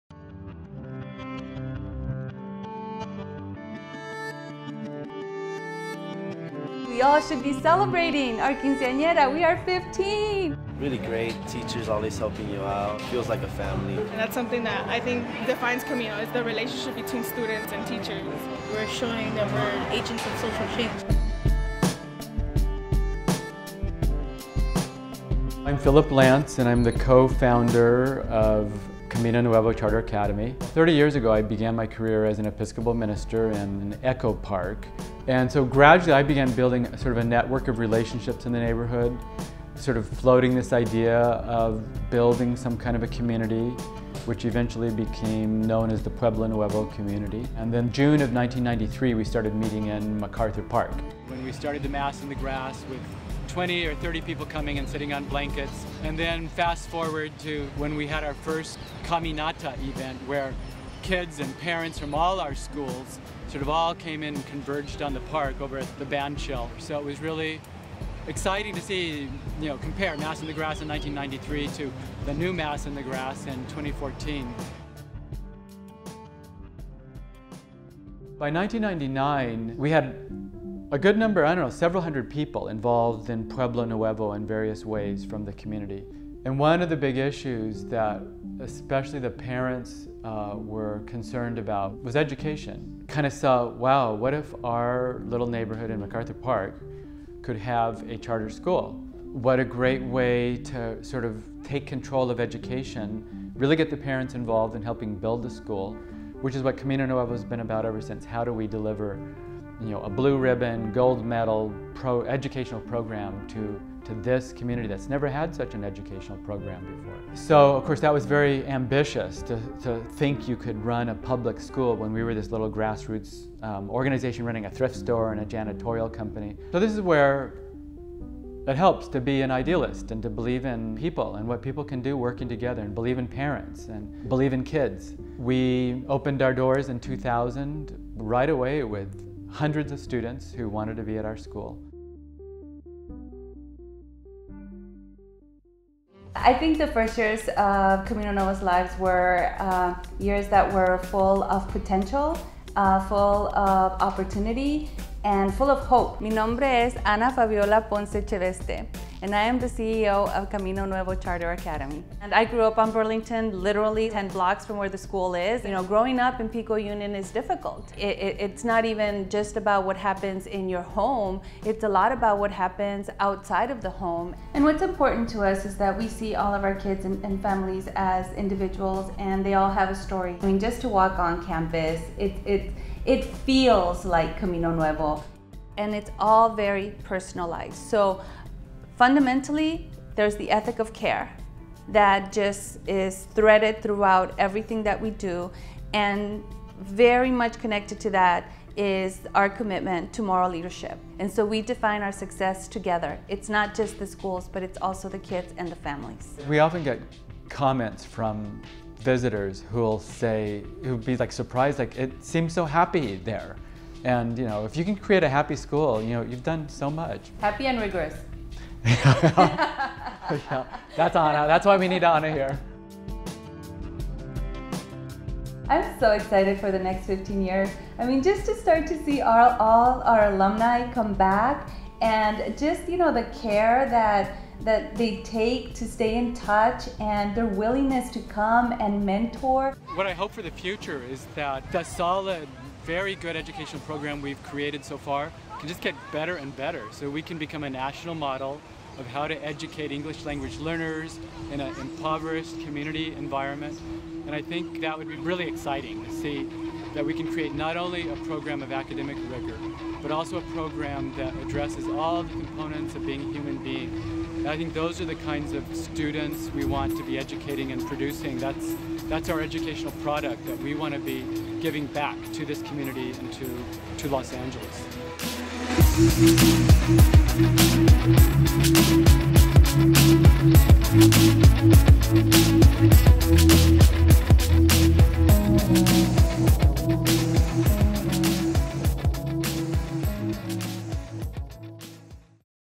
CNCA 15th Anniversary
In celebration of Camino Nuevo Charter Academy's 15th year anniversary, we look at our past, present and future.